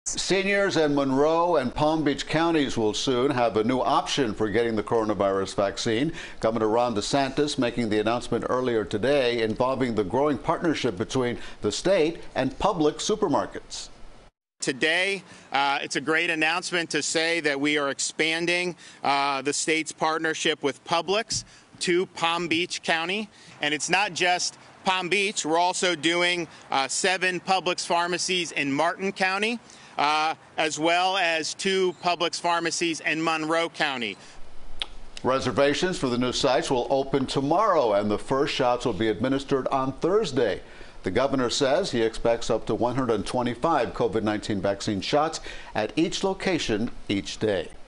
All Publix Stores In Palm Beach County To Offer COVID-19 Vaccinations
The vaccinations are by appointment only, no walk-ins will be accepted.